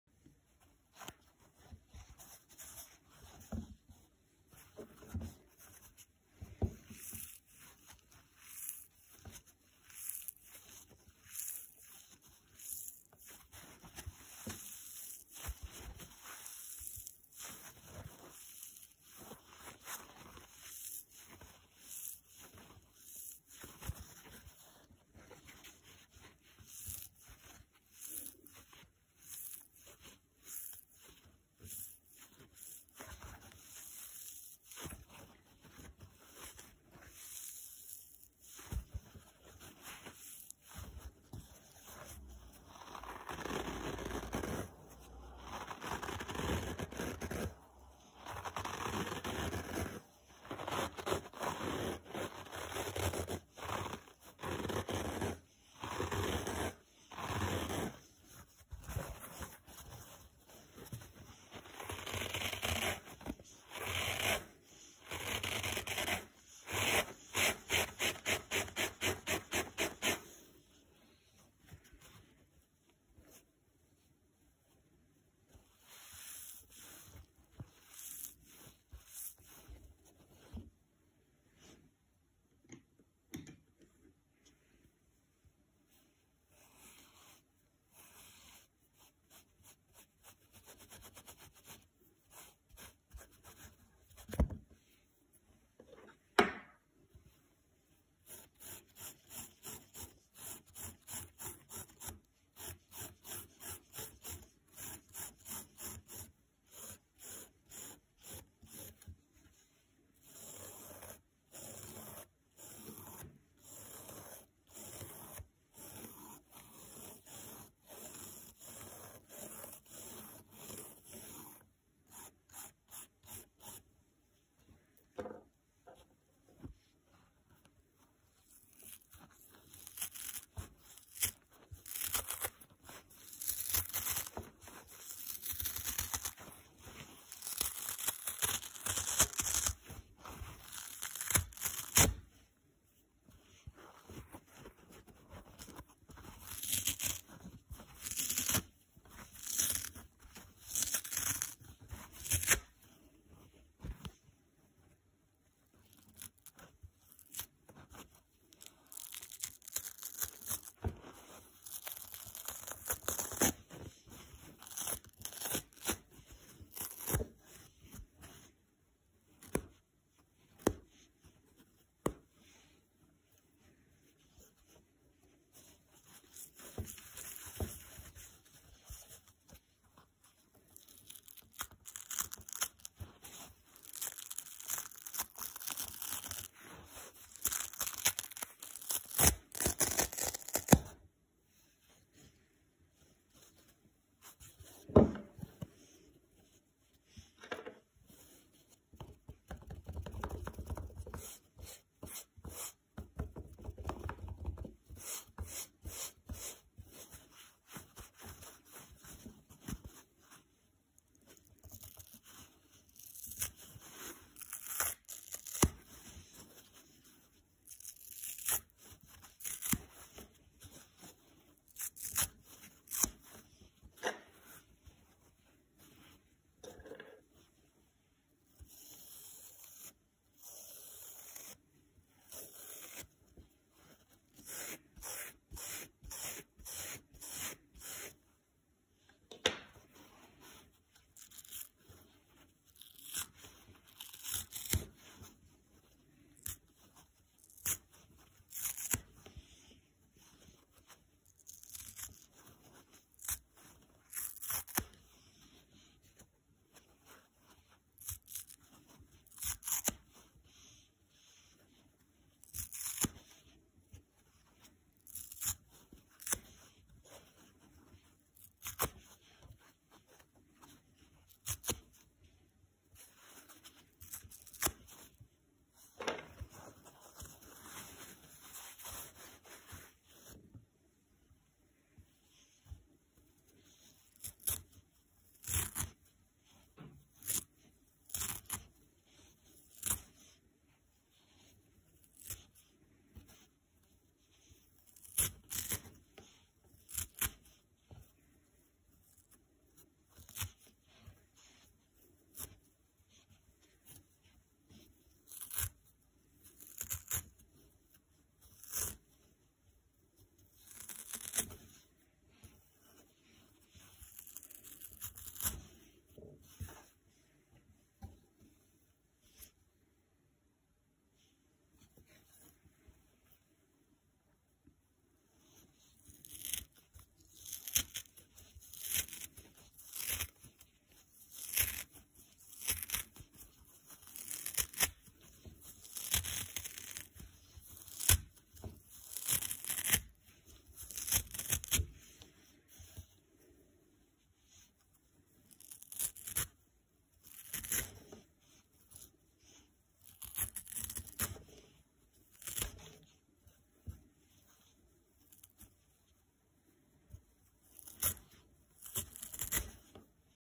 || ASMR || 2x Dry sponges 🧽 super crispy sounds: squeezing, scratching, ripping both scour & soft
00:00 Squeezing
00:42 Scratching
1:24 Makeup Brush Rubbing
1:38 Spoolie Brush Rubbing
2:07 Yellow Sponge scour removal
2:56 Blue Sponge scour removal
3:18 Tapping and scratching soft side
3:30 Soft side ripping
4:43 Scour ripping